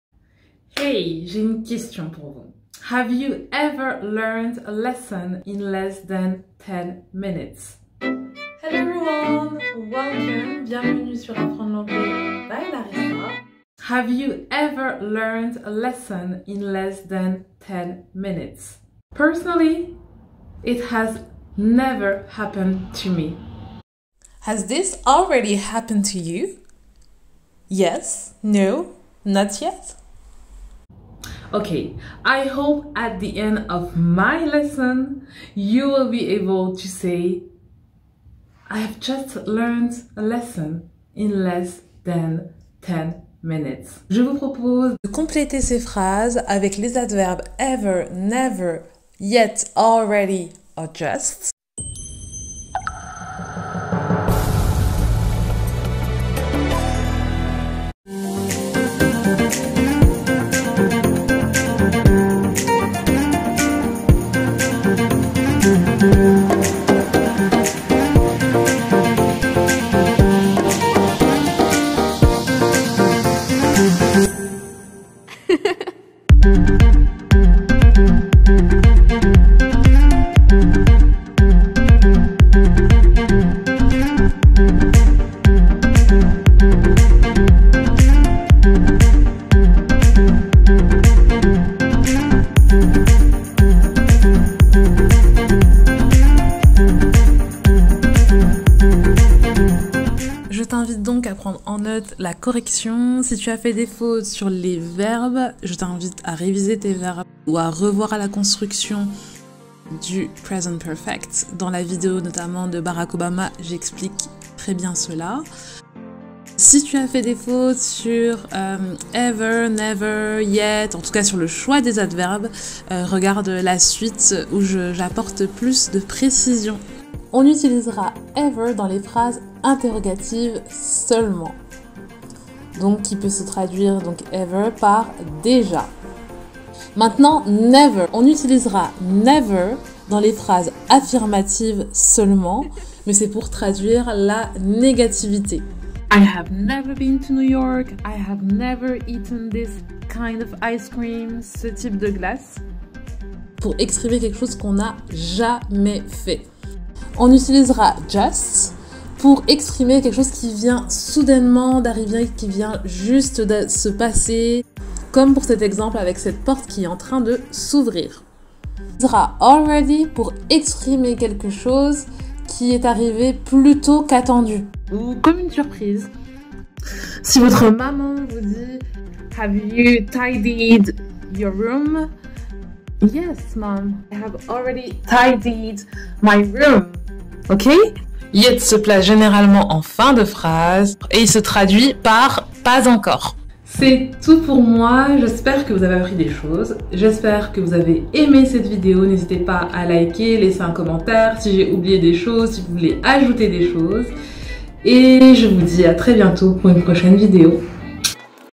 Present perfect (partie 2 ) - mini quizz + correction
Cette vidéo est la partie 2 sur l'apprentissage du present perfect pour l'anglais

1 -  Intro
2 -  Complète avec le bon adverbe : ever / never/  already/ just/ yet 0:55
3 -  Correction à 2:08
4- Récap sur l'usage des adverbes : 2:09